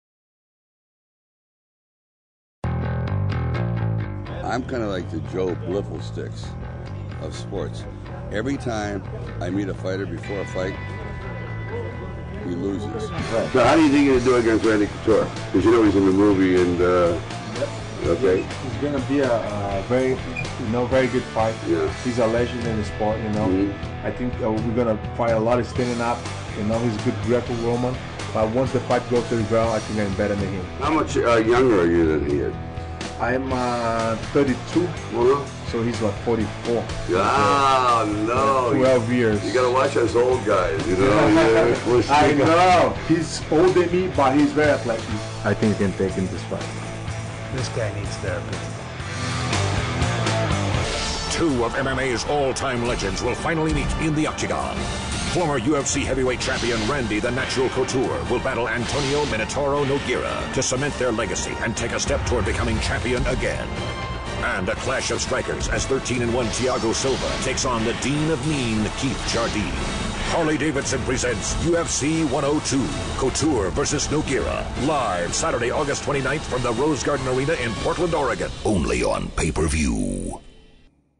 Stallone Gets the Skinny on UFC 102 Couture vs Nogueira
Sylvester Stallone gets the rundown on UFC® 102 straight from Randy Couture and Antonio Minotauro Nogueira on the set of the new movie The Expendables.